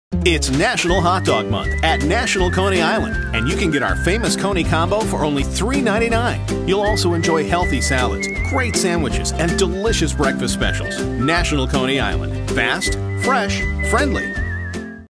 National Hot Dog Month at National Coney Island
It's National Hot Dog Month at National Coney Island and you can get our famous coney combo for only $3.99. You'll also enjoy healthy salads, great sandwiches and delicious breakfast specials. National Coney Island. Fast. Fresh. Friendly.